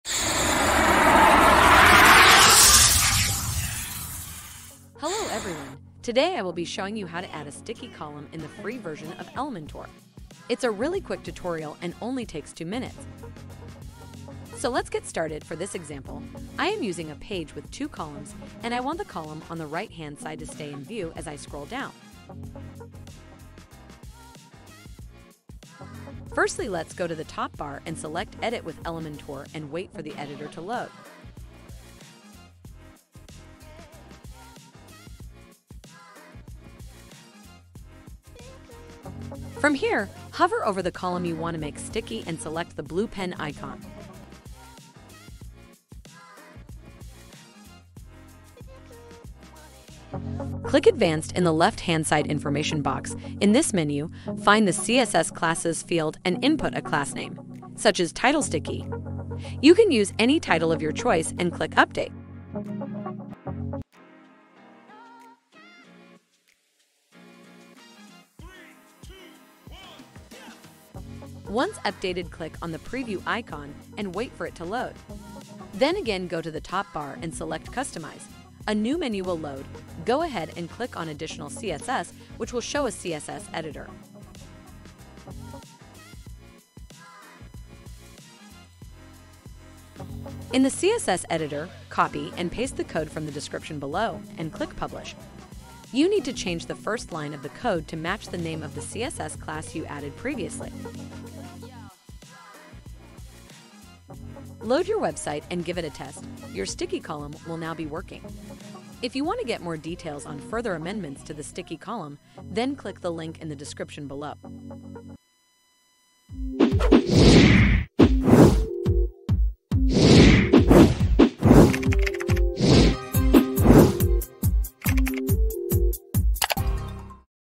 Elementor Tutorial: How to add a Sticky Column to the Free Version of Elementor in 2 Minutes
Are you using the free version of Elementor and want to add a sticky column to your website? In this video, we'll show you how to do it in just a few simple steps. Our easy-to-follow tutorial takes you through the process with a working example, and it's all done in under 2 minutes. Improve the design, functionality and enhance the user experience of your WordPress site with this handy Elementor trick.

Code to Use in the Additional CSS:

.titlesticky{
position: sticky;
position: -webkit-sticky;
top: 3rem;
.parent-sticky
.elementor-widget-wrap {
display: block!important;

Want more tips and tricks on how to improve your website and grow your business?